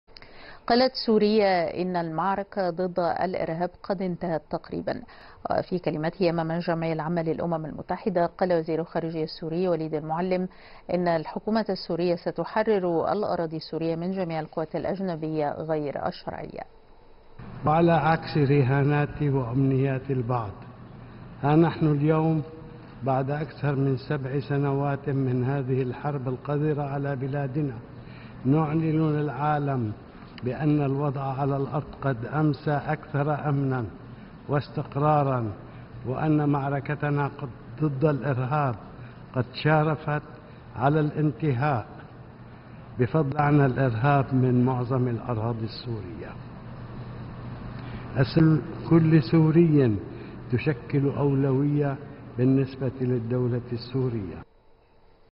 وزير الخارجية السوري: المعركة ضد الإرهاب انتهت تقريبا
قال كبير الدبلوماسيين السوريين للعالم يوم السبت إن "المعركة ضد الإرهاب قد انتهت تقريبا". وفي كلمته أمام الجمعية العامة للأمم المتحدة، قال وزير الخارجية السوري وليد المعلم إن الحكومة السورية ستحرر البلاد من جميع القوات الأجنبية "غير الشرعية".